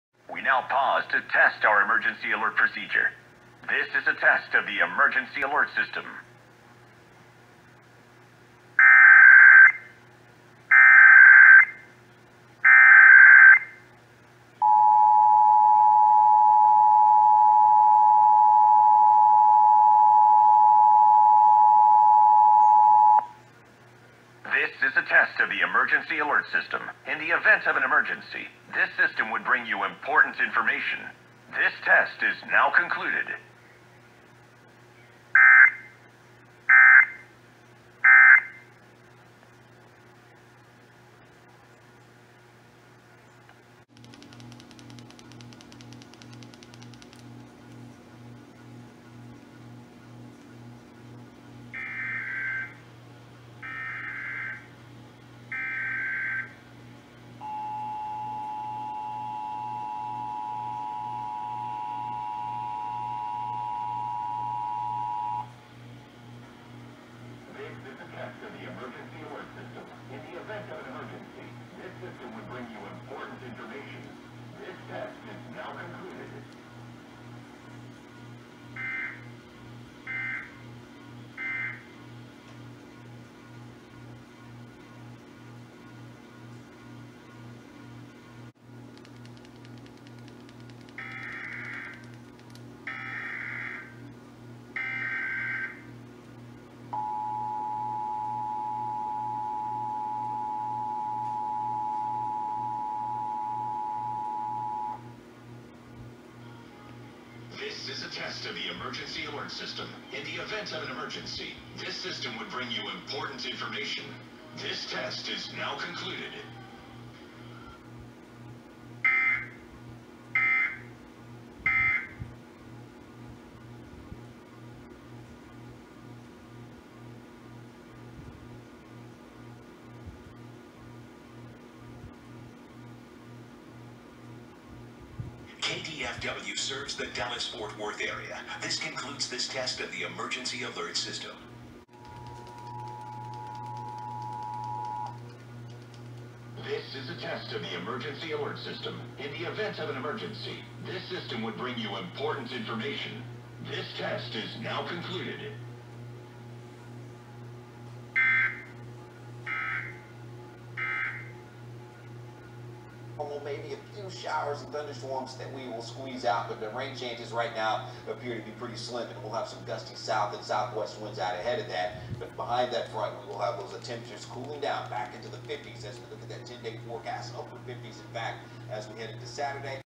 (EAS #101-104) December 2021 Texas Required Monthly Test (WBAP-AM, KXAS-TV, KDFW-TV and WFAA-TV)
This is Texas' required monthly test for December 2021, as seen on WBAP, KXAS, KDFW and WFAA.